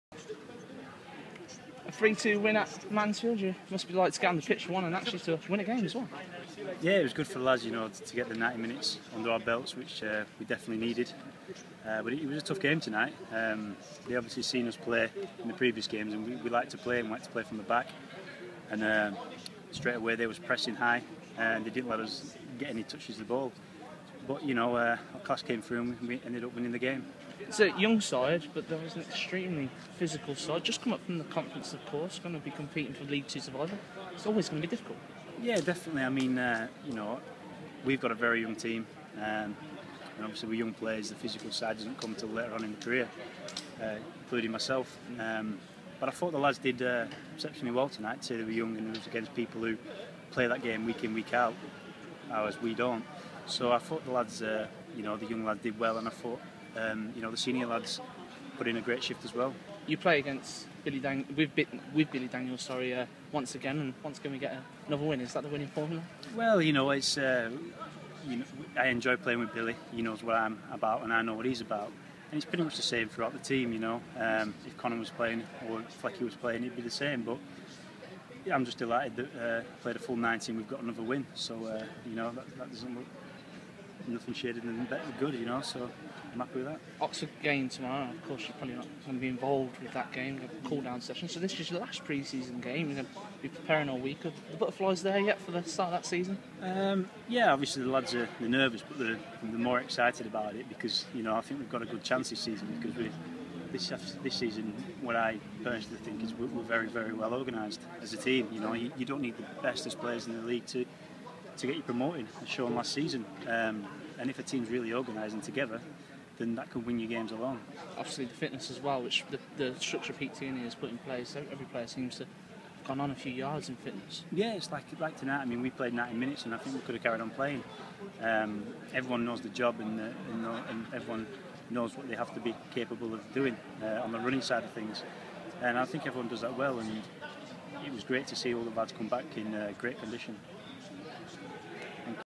Barton on 3-2 win over Mansfield
Coventry City midfielder speaks to Sky Blues Player after friendly win and can't wait for season to start